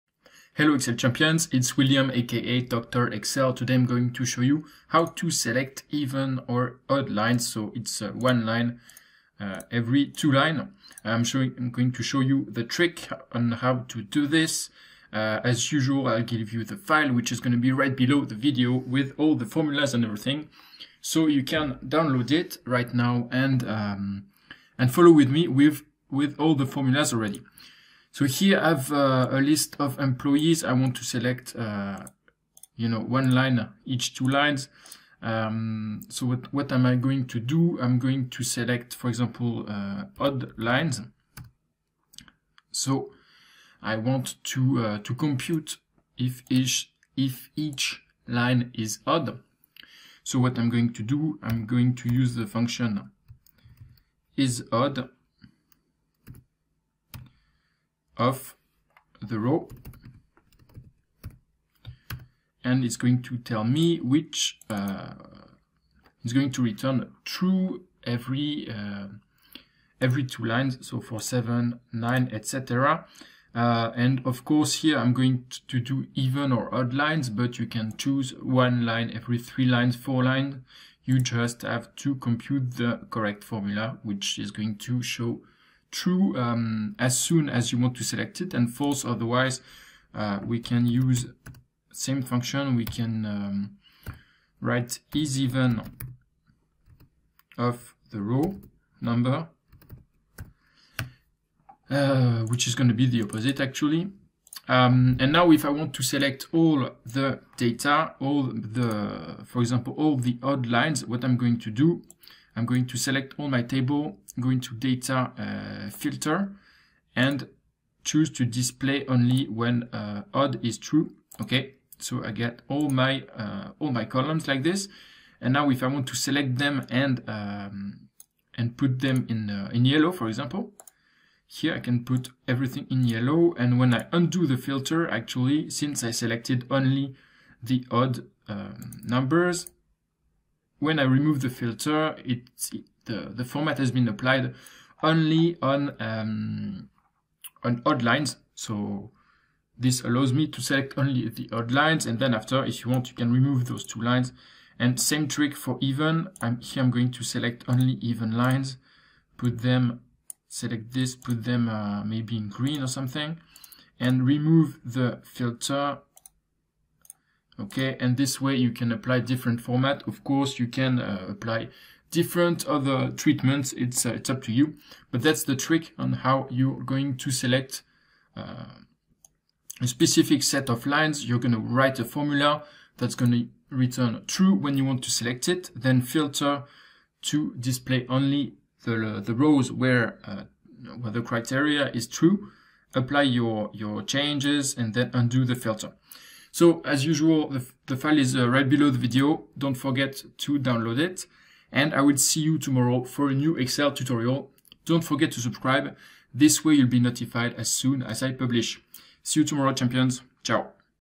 Excel time saver: Select every odd row (or all even rows) - Doctor Excel #086
👇 All bonus info below

Doctor Excel shows you he trick to select all odd rows or all even rows.
This works too if you want to select one in every three rows or any other formula.
You will learn every formula needed and how to apply a specific format